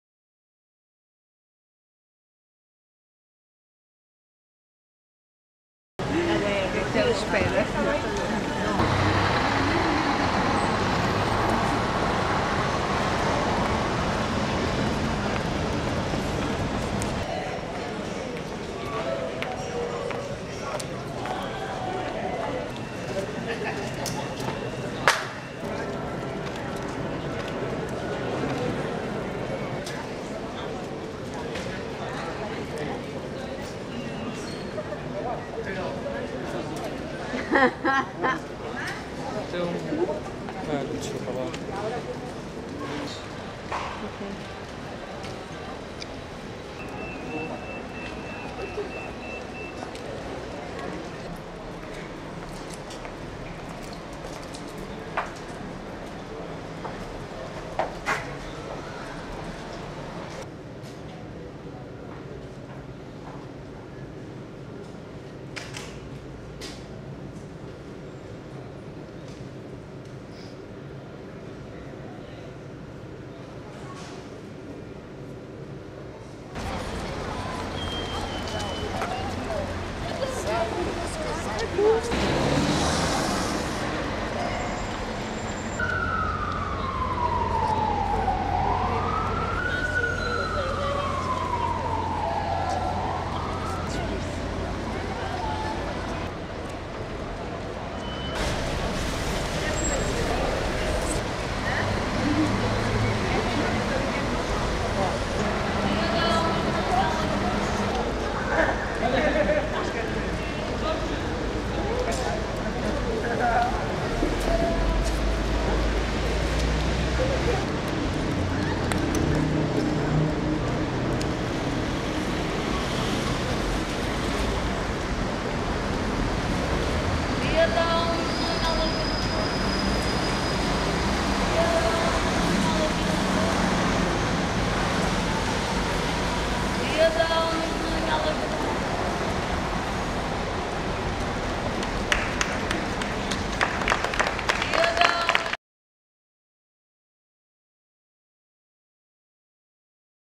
IMERGÊNCIA 2011. Hugo de Almeida Pinho e André Fonseca - Mer Ka Bah
Hugo de Almeida Pinho e André Fonseca (Portugal) - Mer Ka Bah
A performance criará um percurso, com um imaginário que se pode dizer quase místico, encarna-se a expectativa do viajante de sentir o espanto, de se ser veículo da luz numa praça...